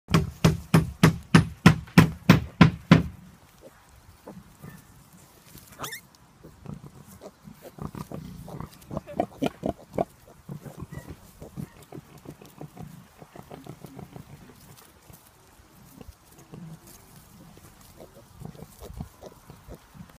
One hour old saddleback pigs
What a surprise when we went to feed the pig this morning, we thought mum was three week away from giving birth.